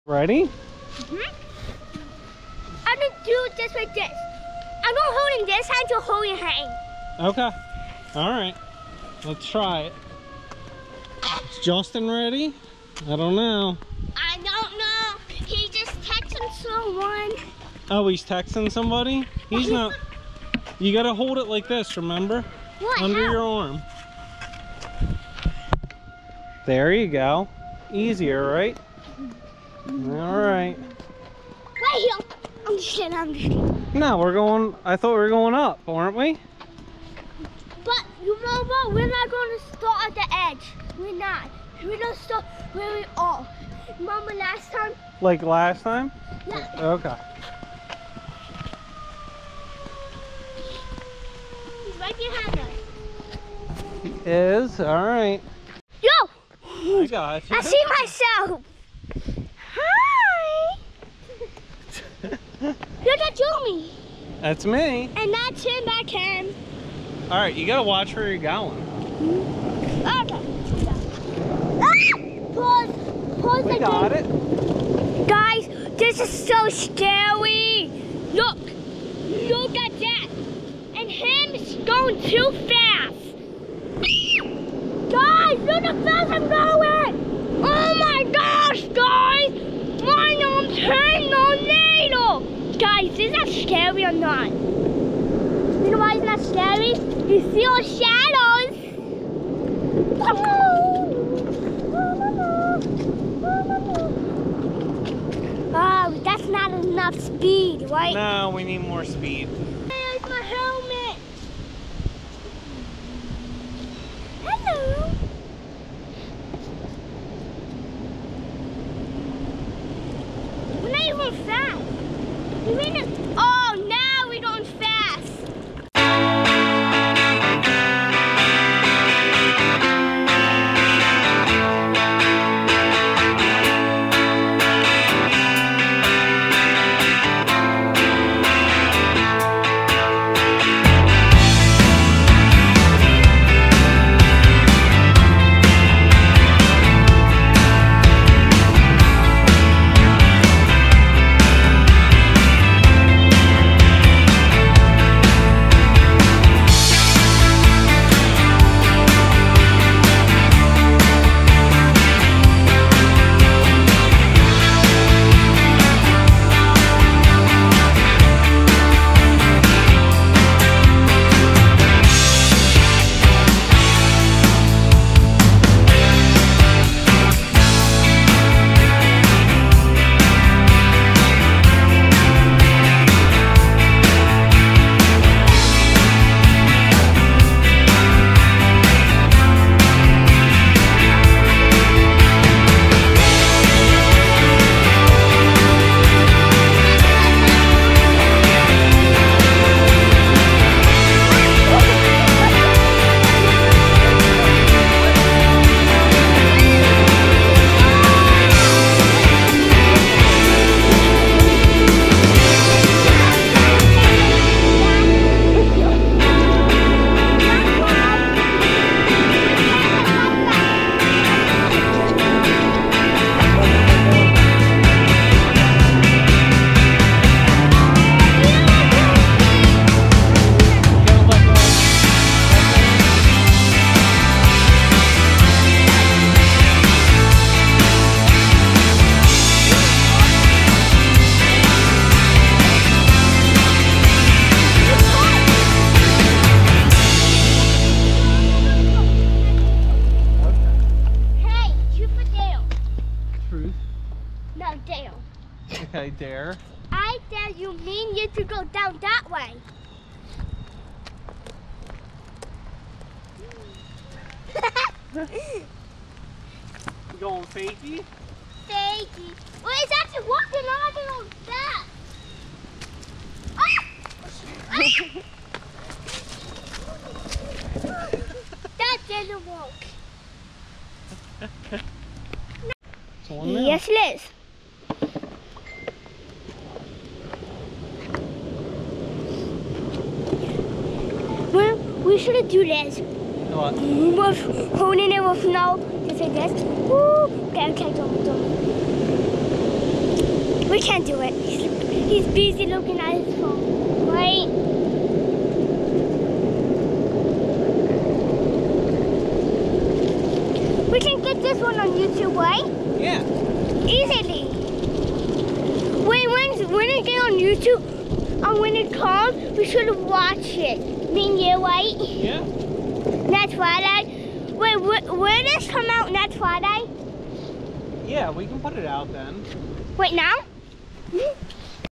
Cooper Skating his Custom Board
Cooper Skating his Custom Board down the "Mountain" ; )